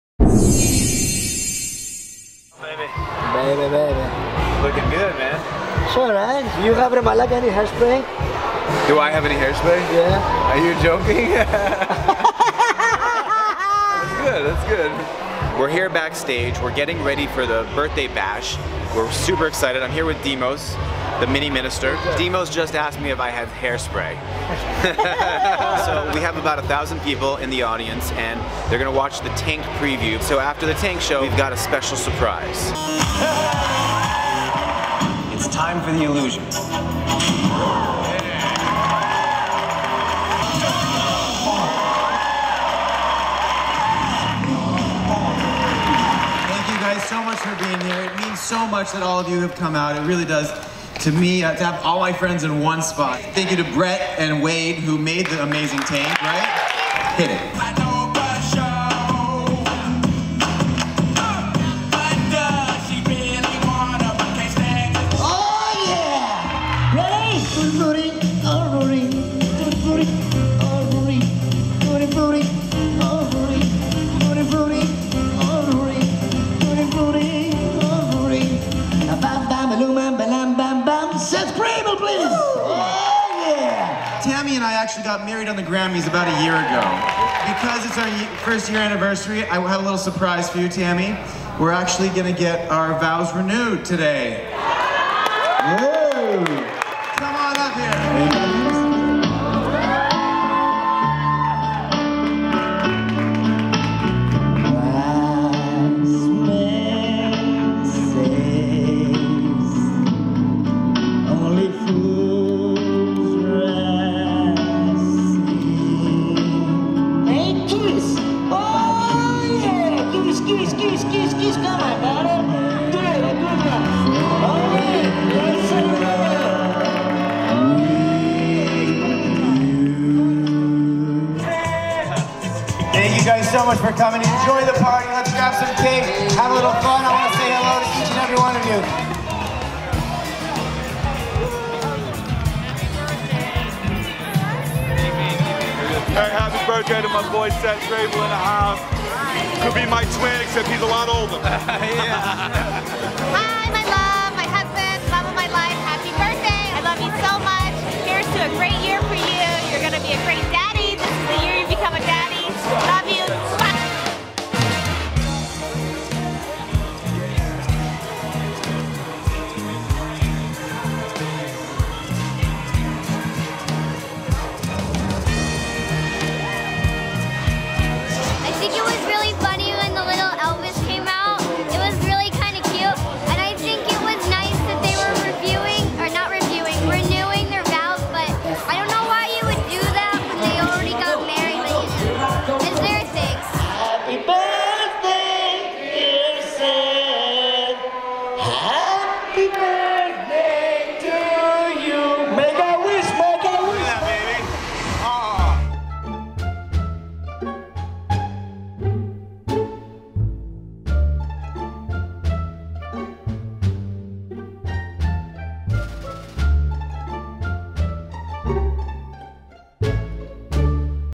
Seth Grabel's Birthday Bash
Seth celebrates his birthday in style by presenting a special surprise to his wife Tammy.